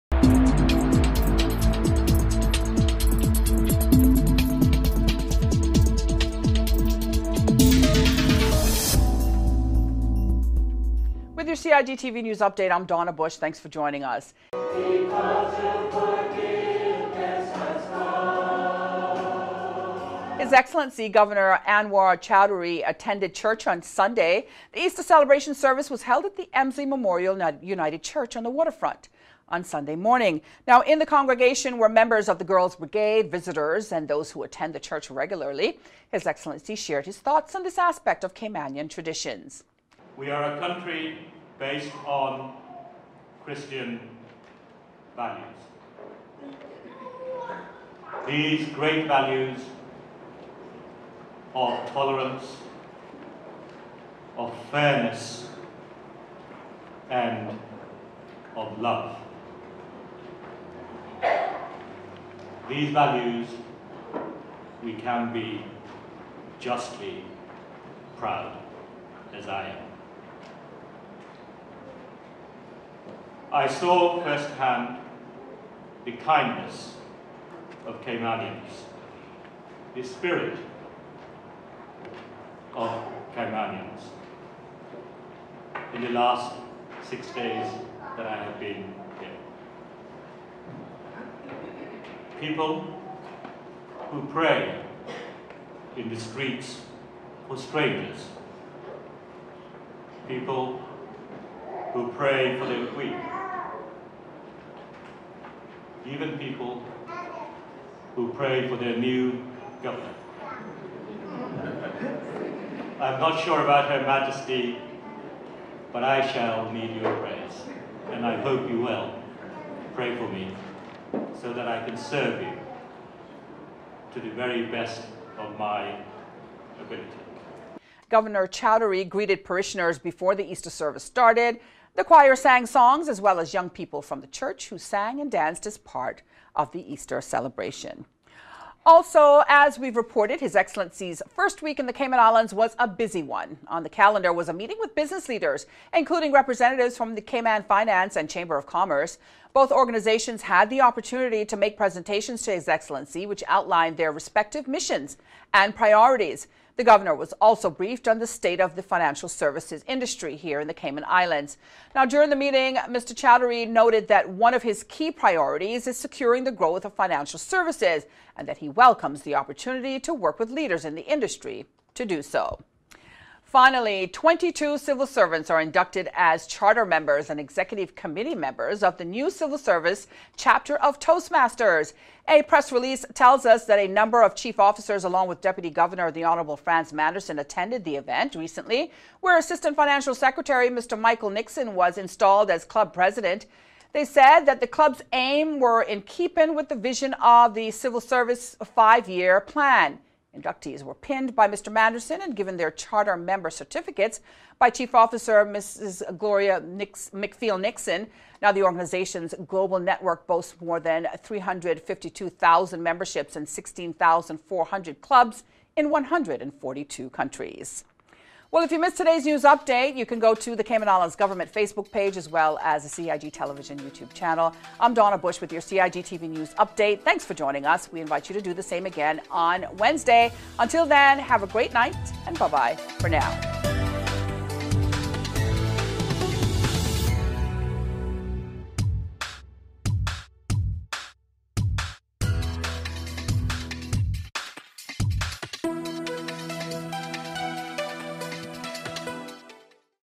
CIGTV News Update 1264, April 3 2018
Making our CIGTV news update headlines, His Excellency Governor Choudhury attends Easter service at Elmslie Memorial United Church. Governor Choudhury hears from business leaders, and Civil Servants form a new Toastmasters chapter.